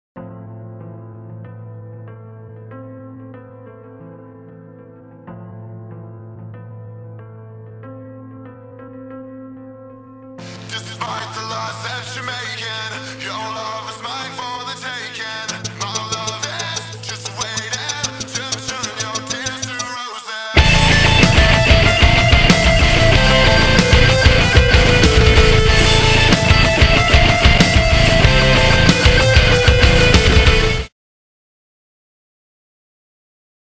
D A R K | t e a r s 2 r o s e s
hello minna!
so this is my entry for playingxfavorite's visual aesthetics competition. Its a lil late, and im really sorry for that, but its all good! im glad i am still able to participate... although i dont think ill make it much farther ^^" im really happy i learned to do something new though, this was so out of my comfort zone! info at the end of video.